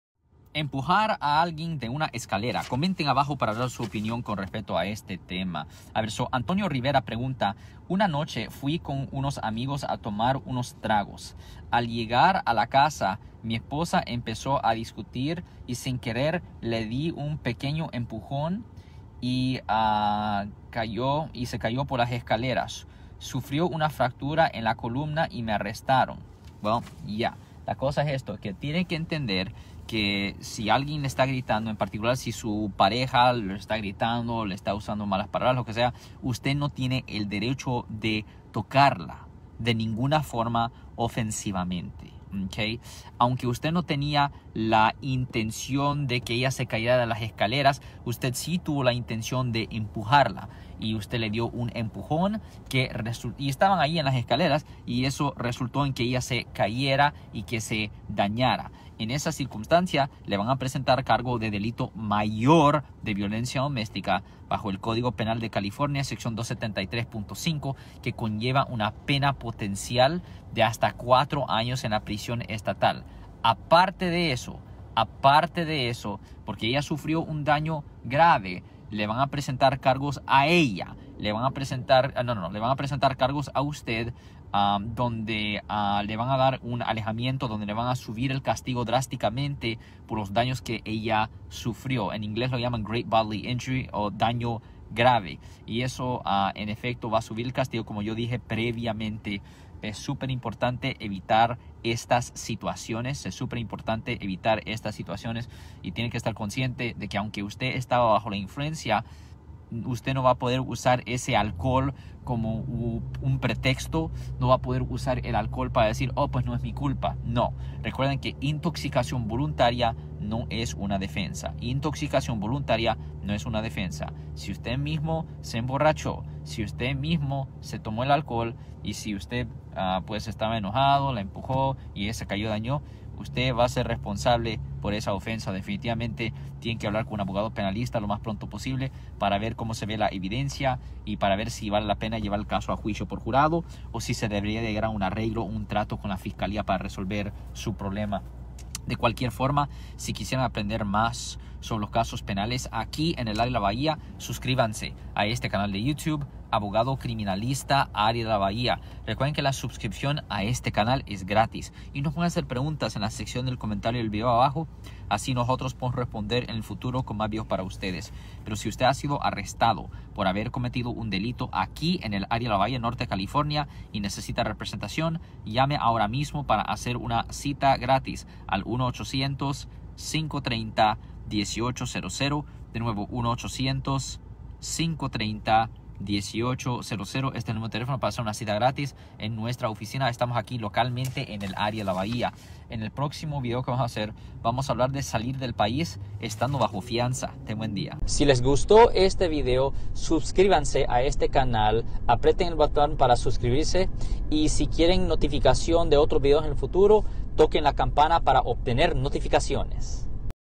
EMPUJAR A ALGUIEN DE UNA ESCALERA
UNA NOCHE FUI CON UNOS AMIGOS A TOMAR UNOS TRAGOS, AL LLEGAR A LA CASA, MI ESPOSA EMPEZÓ A DISCUTIR Y SIN QUERER LE DI UN PEQUEÑO EMPUJÓN Y CAYÓ POR LAS ESCALERAS, SUFRIÓ UNA FRACTURA EN LA COLUMNA Y ME ARRESTARON, NO SE COMO DEFENDERME.
“ANTONIO RIVERA" PREGUNTA;